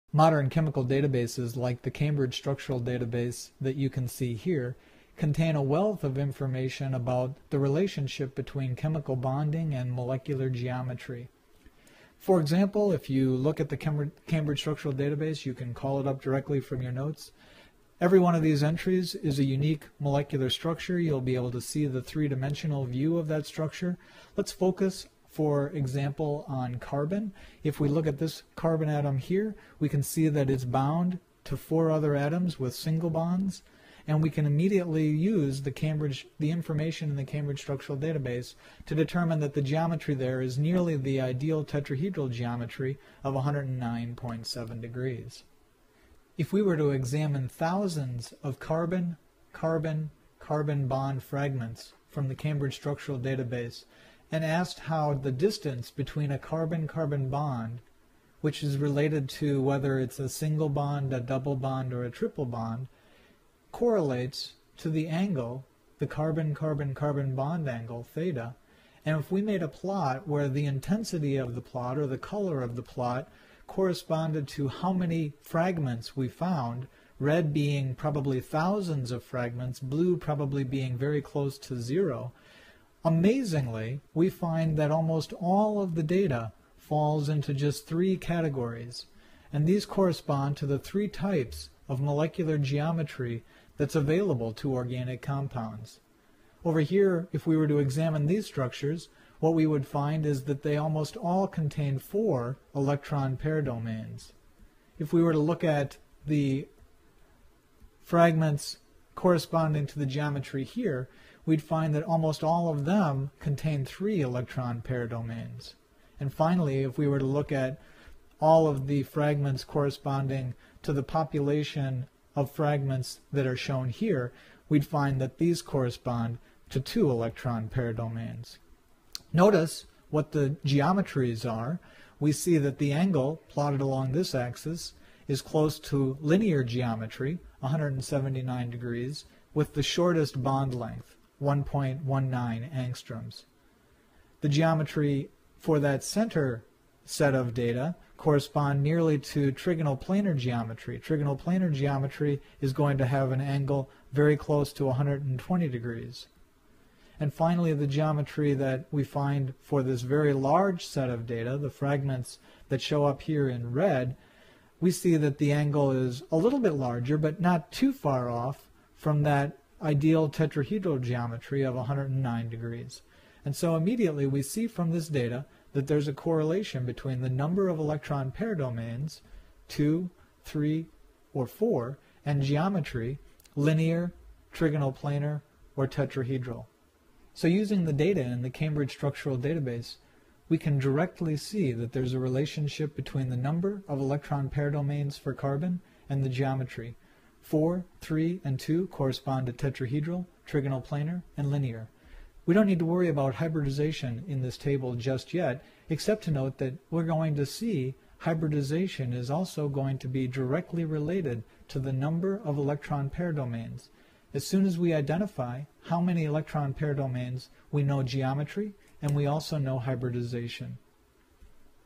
Geometry Depends on the Number of Electron Pair Domains
(External link to the Cambridge Structural Database)
In a molecule, each atom's geometry correlates to the number of electron pair domains surrounding that atom.  This is a statement of the valence shell electron pair repulsion model.  The conclusion is supported by data in the Cambridge Structural Database.